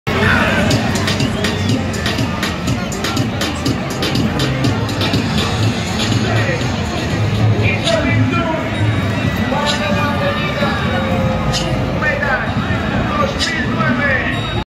Feria de la Pradera de San Isidro 2019 en Madrid.
San Isidro 2019 en Madrid la víspera del dia de romería.